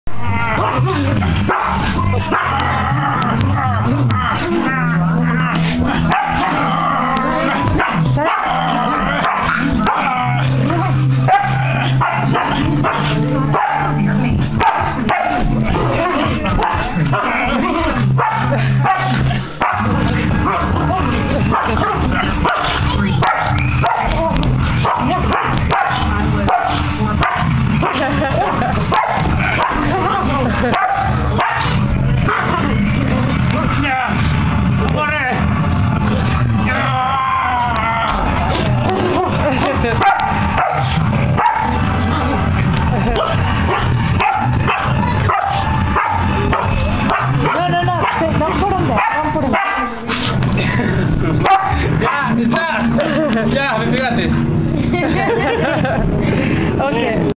DOG FIGHT- Samoyed Vs ...
This is not a fight ppl. This is my bro playing with Teddy Bear my 1 yr old samoyed.
Teddy was not hurt and was not thrown for real watch as he is put gently down. They are just playing.